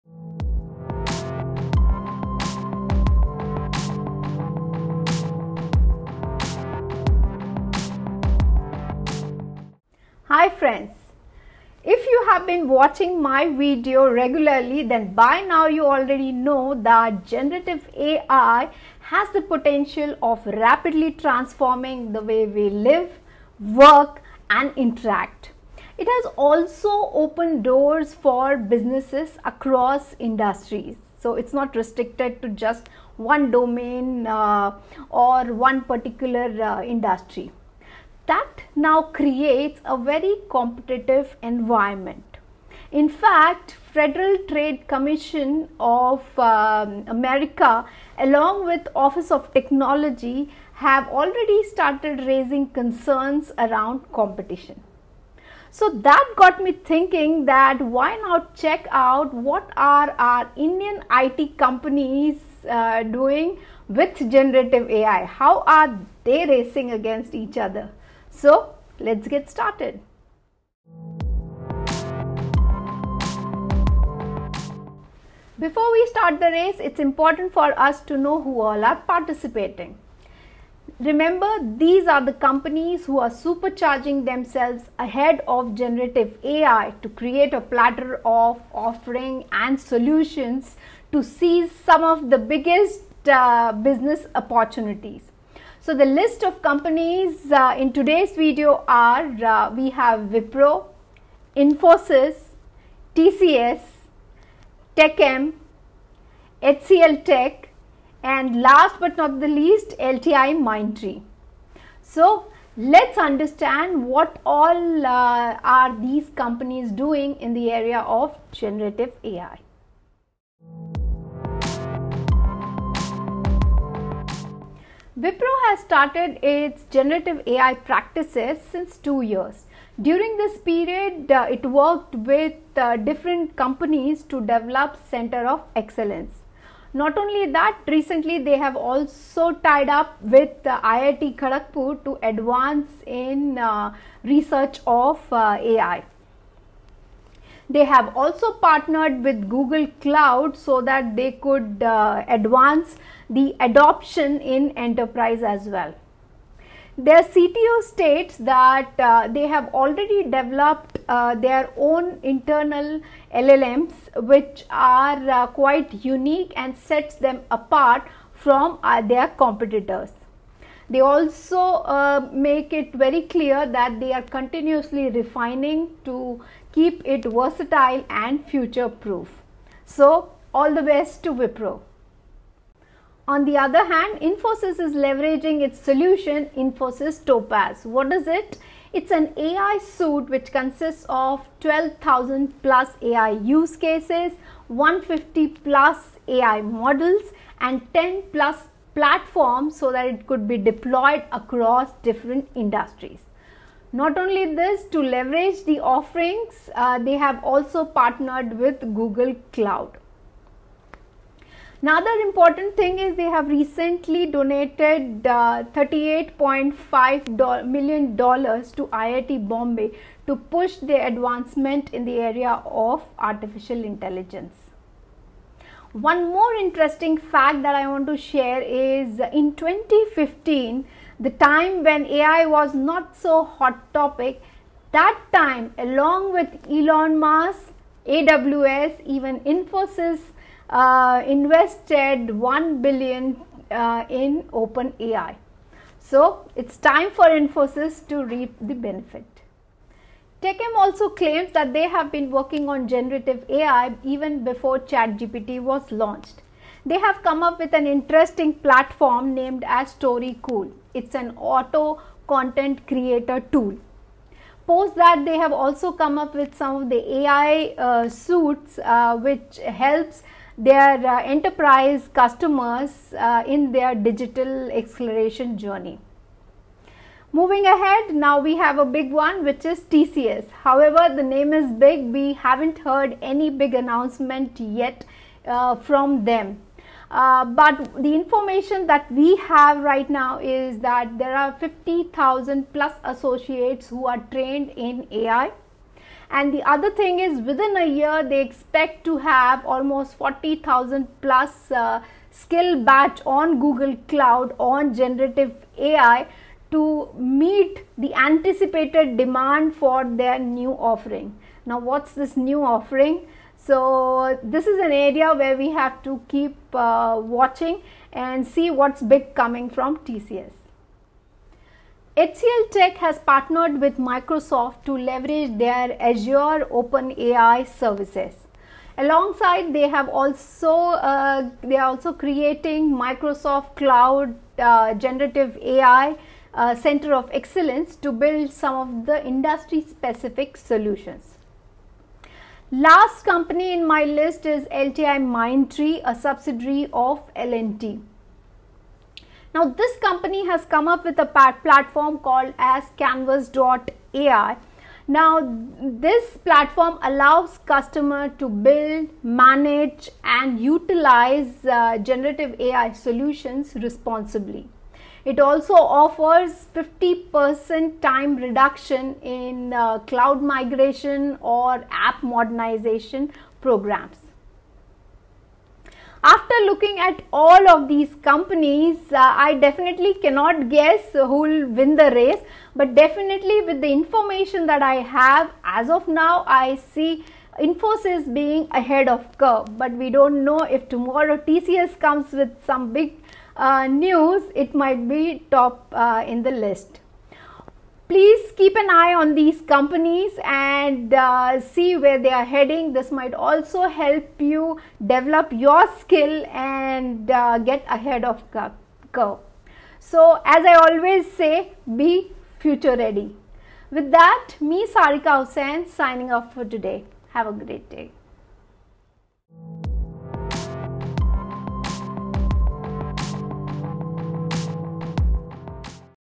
Video #54 Indian IT Generative AI Race Begins
Let's checkout, how Indian IT companies competing on Generative AI? This video uncovers multiple applications and efforts taken by these companies to advance in this field.